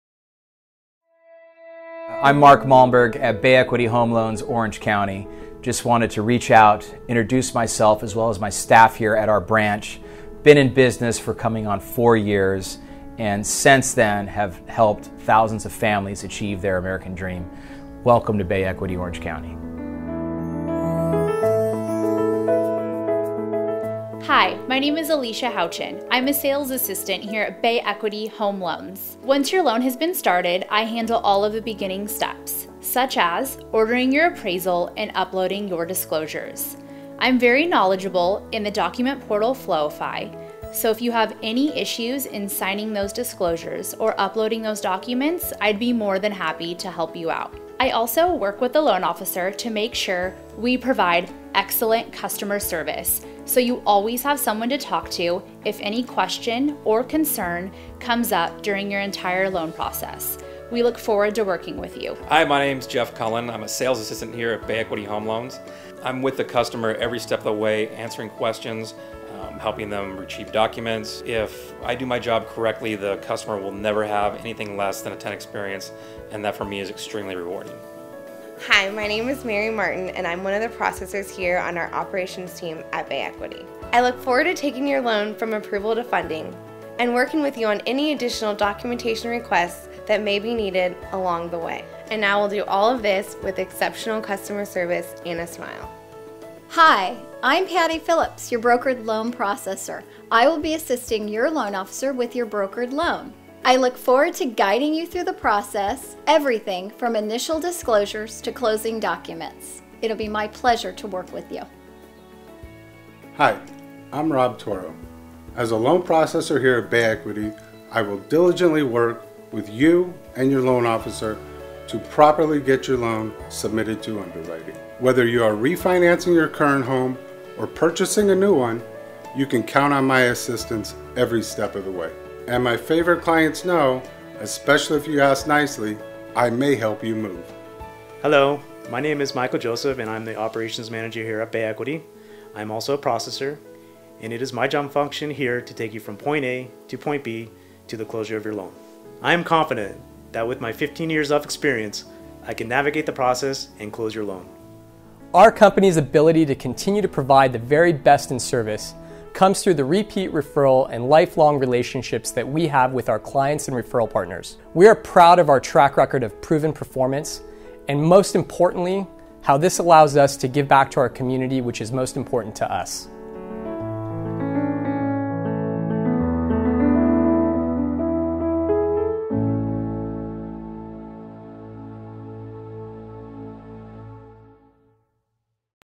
Bay Equity Welcome
Bay Equity Home Loans in Orange County has helped thousands of families achieve their home dreams.  With this video you have a chance of meeting the wonderful people who help make those dreams come true.